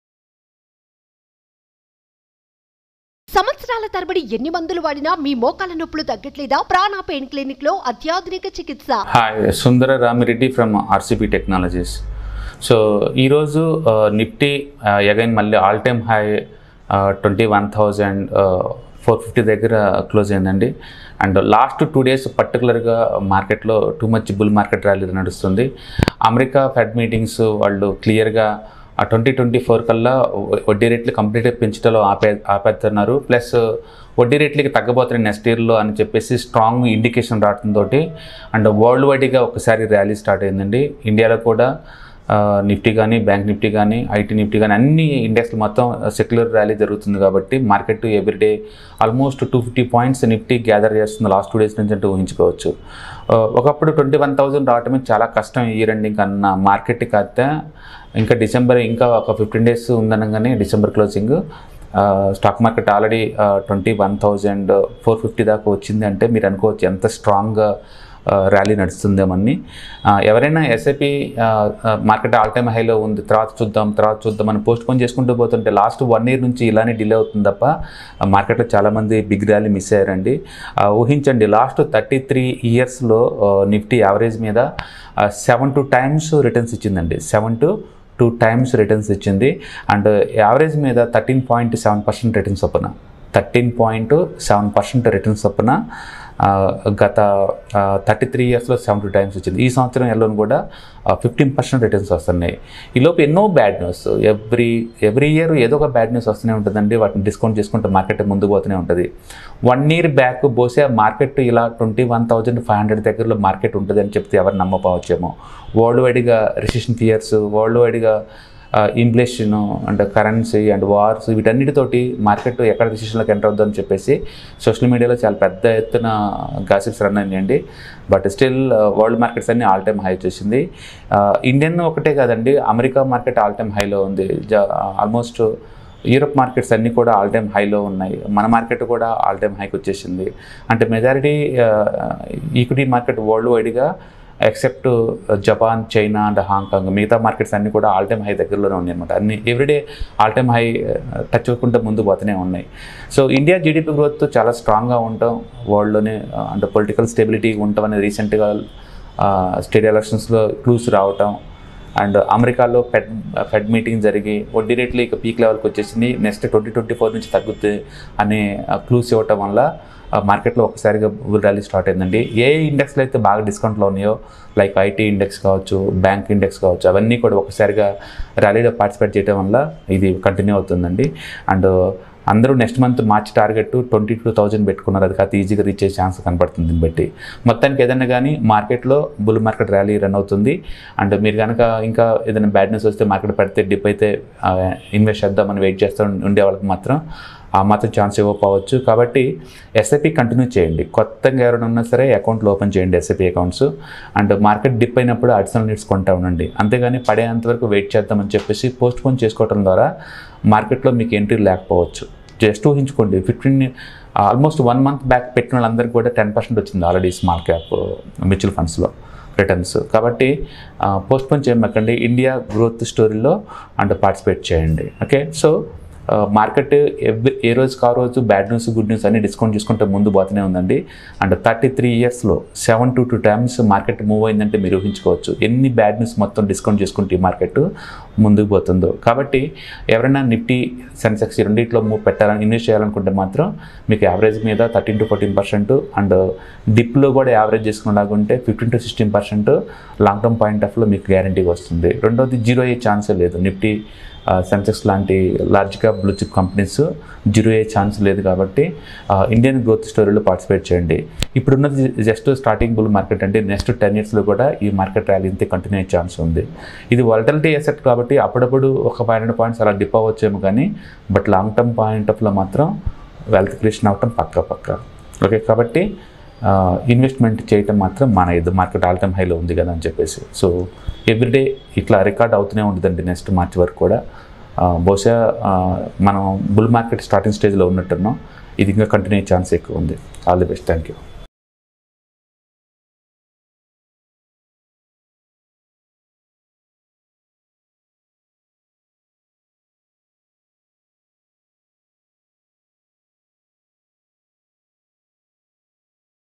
Stock Market All Time High Bull Rally | 72X Growth in NIFTY 50 Growth History | SumanTV Money
Watch▶ Sundara Ramireddy - Stock Market All Time High Bull Rally | 72X Growth in 33 years! NIFTY 50  Growth History | SumanTV Money